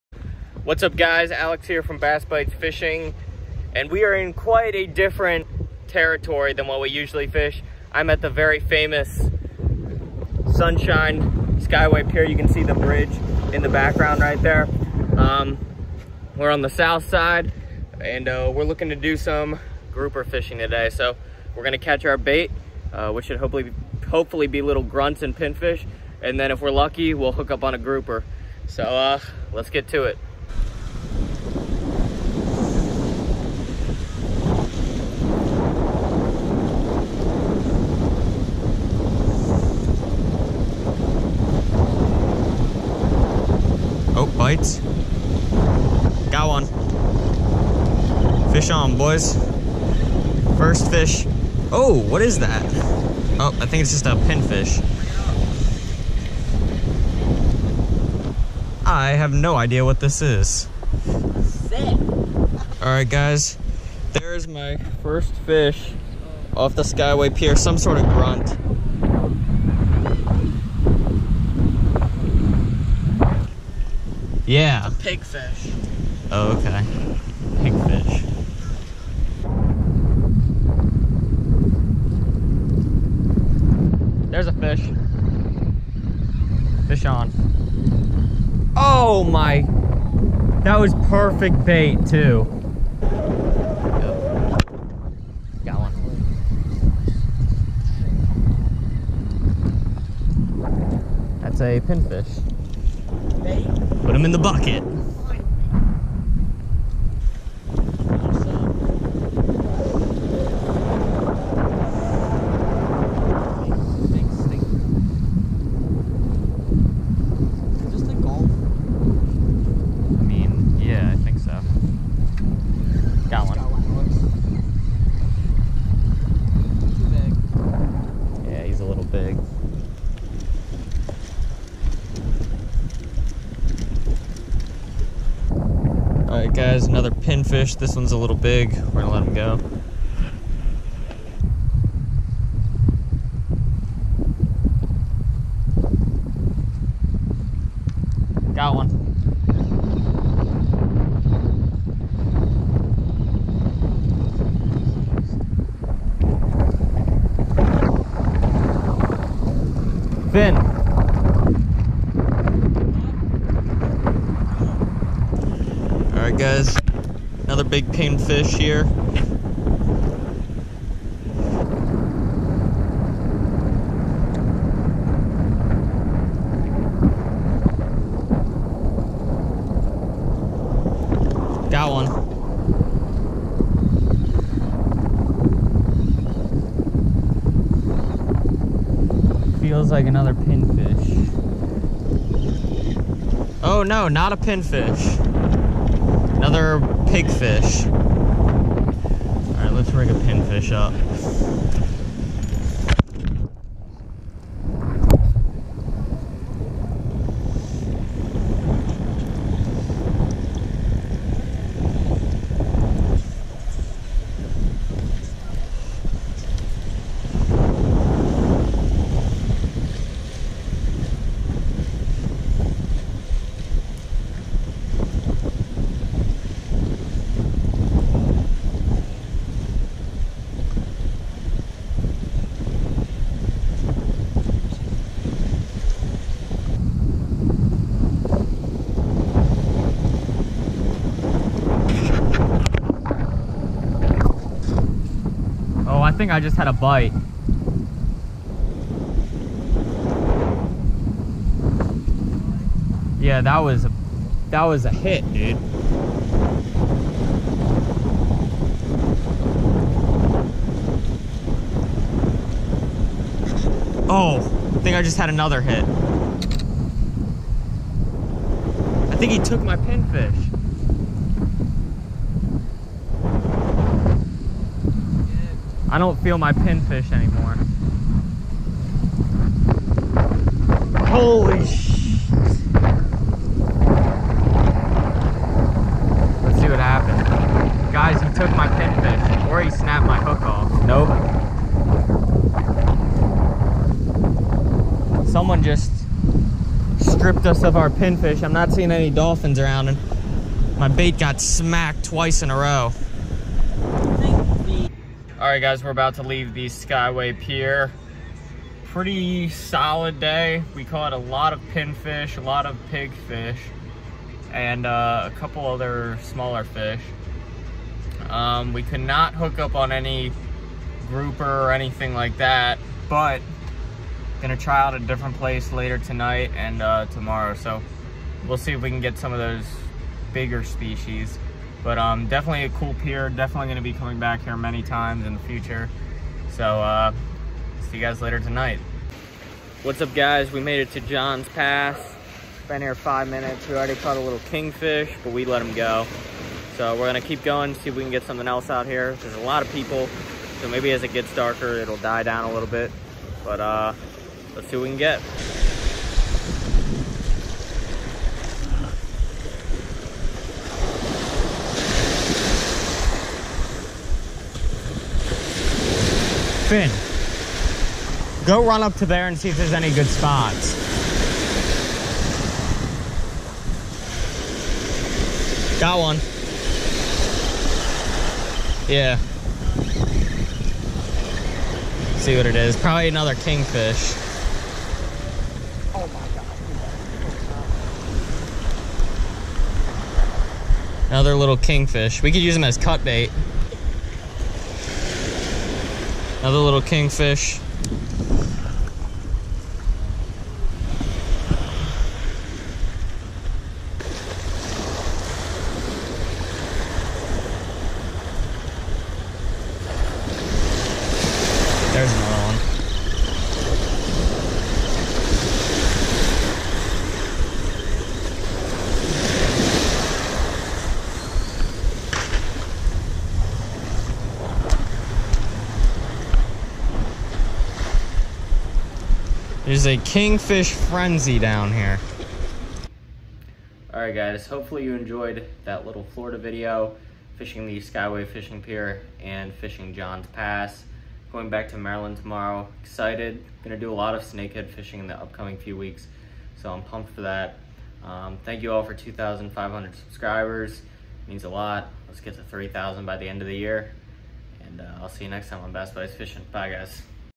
Fishing the Skyway Pier and Johns Pass | Madiera Beach Florida Fishing (Pinfish, Pigfish, Kingfish)
In this Florida video, I fish the very famous Sunshine Skyway Pier, and also Johns Pass in Madiera Beach Florida using shrimp.